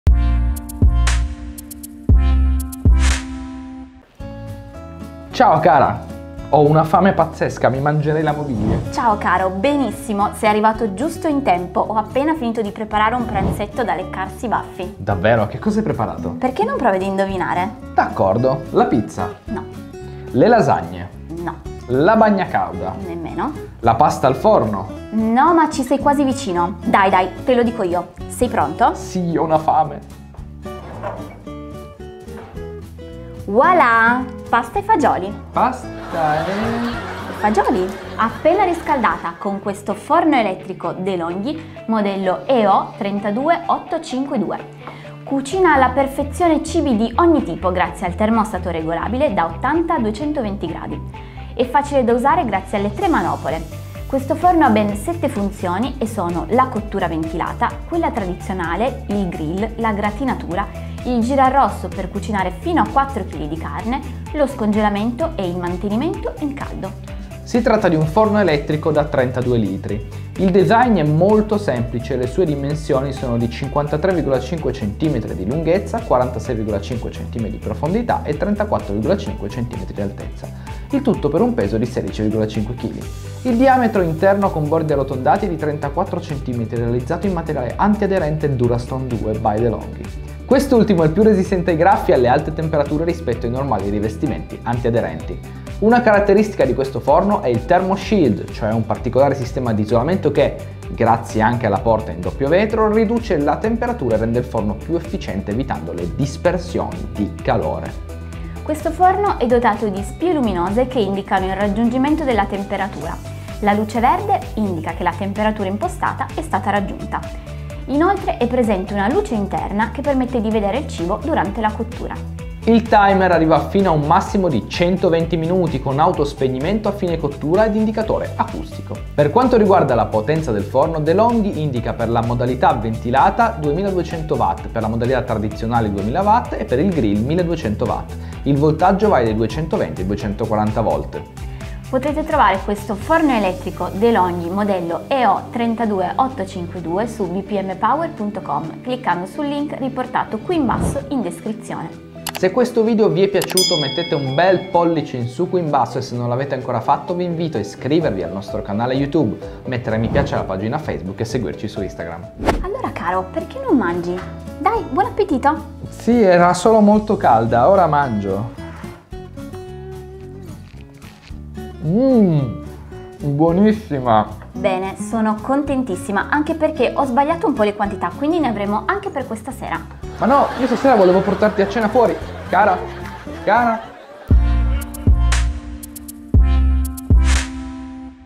Forno elettrico DeLonghi Sfornatutto EO 32852, pratico per casa e oltre!
Diamo un'occhiata al Forno elettrico DeLonghi Sfornatutto EO 32852, che cucina alla perfezione cibi di ogni tipo grazie al termostato regolabile da 80 a 220°C. Facile da usare grazie alle tre manopole, questo forno ha ben 7 funzioni e sono la cottura ventilata, quella tradizionale, il grill, la gratinatura, il girarrosto (per cucinare fino a 4 kg di carne), lo scongelamento e il mantenimento in caldo.